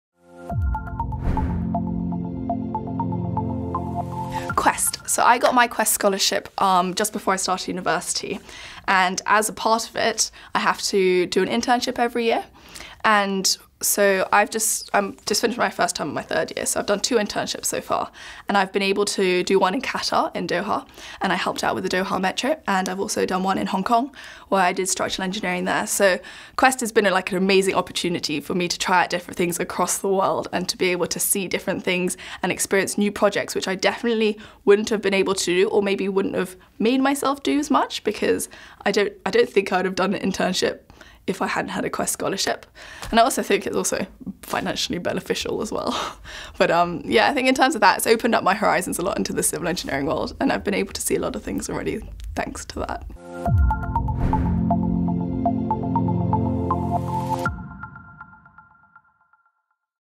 ICE Talks: QUEST Scholarship - Susie McAllister
I got my QUEST scholarship just before I started university and as part of it I have to do an internship every year. I have done two so far, I've done one in Doha and one in Hong Kong, so QUEST has been an amazing opportunity for me to experience things across the world. Do things I may not have been able to do or maybe make myself do so much. I don't think I would have done an internship if I hadn't had a QUEST scholarship. In terms of QUEST its really opened up my eyes to the civil engineering world.

If you would like to explore a career in Civil Engineering